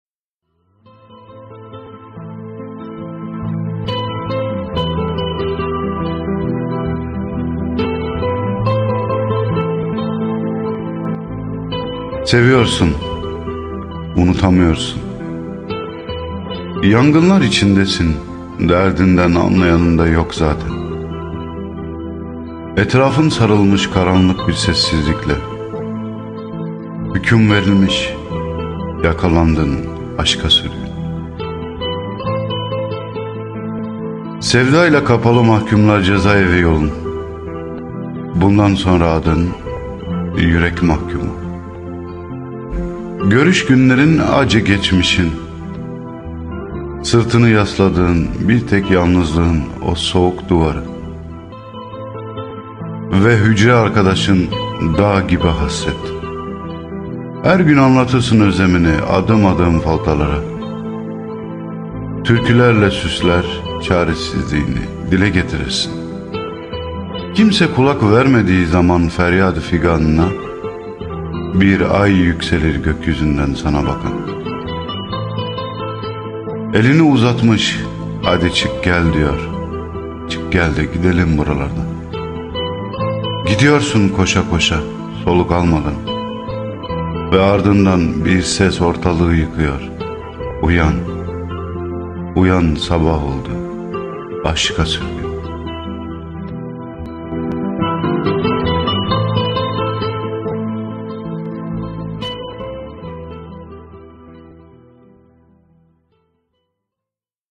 Aşka Sürgün (ŞİİR) Acı Gerçekler AŞK Şiirleri Duygusal Fon Müziği Yaman Karaca
Aşka sürgün

Seviyorsun unutamıyorsun
Yangınlar içindesin derdinden anlayan zaten yok
Etrafın sarılmış karanlık bir sessizlikle
Hüküm verilmiş yakalandın aşka sürgün
sevdaya kapalı mahkümlar ceza evi yolun
Bundan sonra adın yürek mahkumu
Görüş günlerin acı geçmişin
Sırtını yasladığın bir tek yalnızlığın soğuk duvarı
Ve hücre arkadaşın dağ gibi hasret
Hergün anlatırsın özlemini adım adım foltalara
Türkülerle süsler caresizliğini dile getirirsin
Kimse kulak vermediği zaman feryadı fıganına
Bir ay yükselir gök yüzünde sana bakan
Elini uzatmış hadı cık gel diyor gidelim buralardan
Gidiyorsun koşa koşa soluk almadan
Ve artından bir ses ortalığı yıkıyor
Uyan sabah oldu yürek sürgünü...

Ayrıca Mucizem kitabımız da sitemizde  satışa başlamıştır...

Şiir : @yamansiir
Seslendirme :@yamansiir
İnstagram hesabımız : @yamansiir
tiktok hesabımız : @yamansiir_